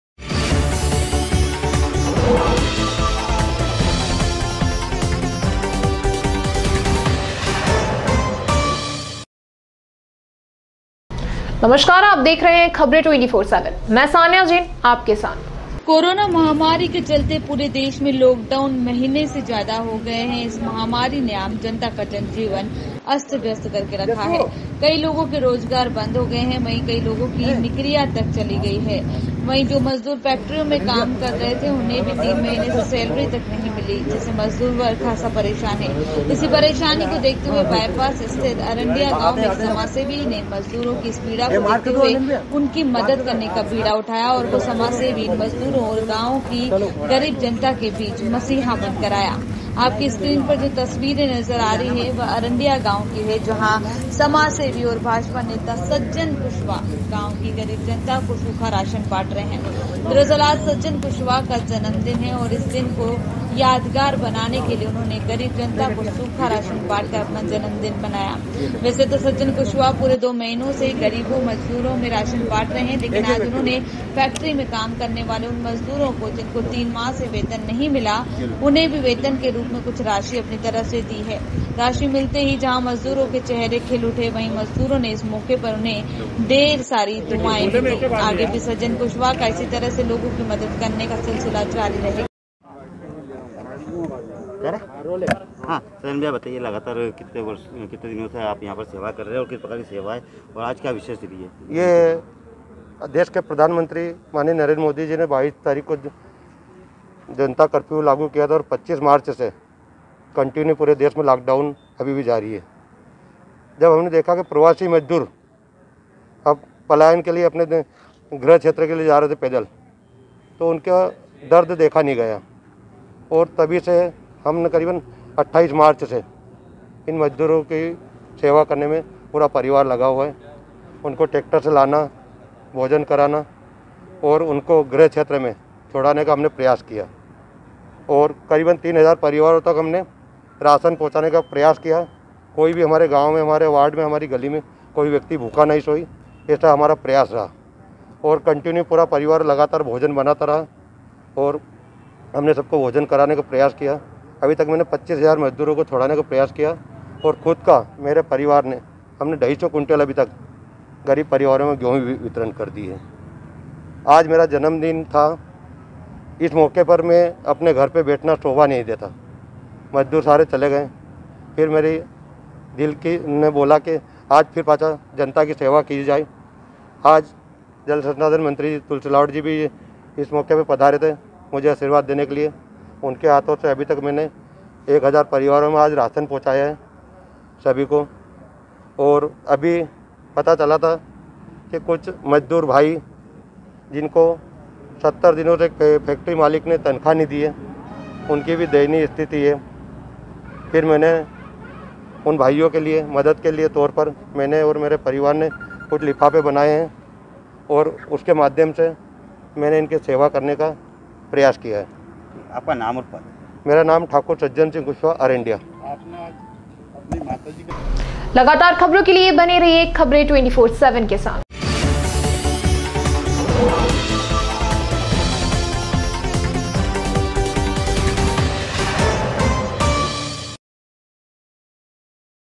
Khabren24x7 सज्जन कुशवाह का जन्मदिन गरीब जनता को सूखा राशन बांटकर अपना जन्मदिन बनाया 08.06.2020
सज्जन कुशवाह का जन्मदिन गरीब जनता को सूखा राशन बांटकर अपना जन्मदिन बनाया